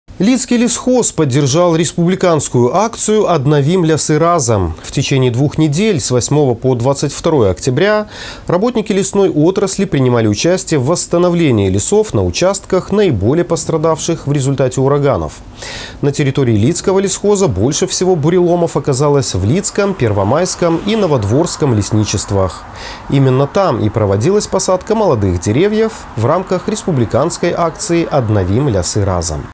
Работники Лидского лесхоза приняли участие в акции «Аднавім лясы разам»
Лидский лесхоз поддержал республиканскую акцию «Аднавім лясы разам». В течение 2 недель (с 8 по 22 октября) работники лесной отрасли принимали участие в восстановлении лесов на участках, наиболее пострадавших в результате ураганов. На территории Лидского лесхоза больше всего буреломов оказалось в Лидском, Первомайском и Новодворском лесничествах. Именно там и проводилась посадка молодых деревьев в рамках республиканской акции «Аднавім лясы разам».